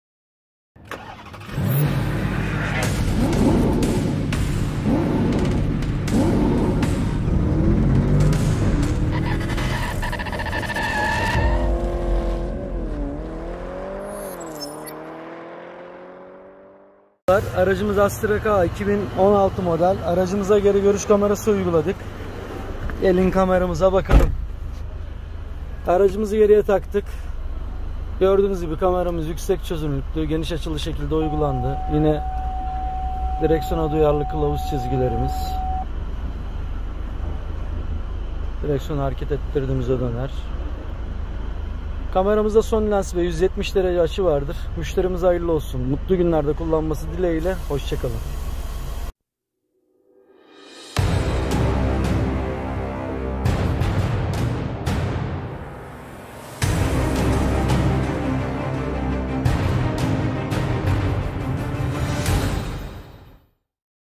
Astra K Geri Görüş Kamerası Montajı
*Opel astra k geri görüş kamerası* ve orjinal programlama uygulaması.
*Opel astra k geri görüş kamerası* işlemi sağlanırken aracınızın mevcut yazılımına ve donanımına herhangi bir zarar vermeden orjinal kodlamalar ile aktif edilmektedir. -Astra K Geri Görüş Kamerası* işleminde geniş görüş açılı 170° Sony lensi kamera kullanılmaktadır. *Astra K Geri Görüş Kamera Kurulum* işlemleri için bizlerden detaylı bilgi alabilirsiniz.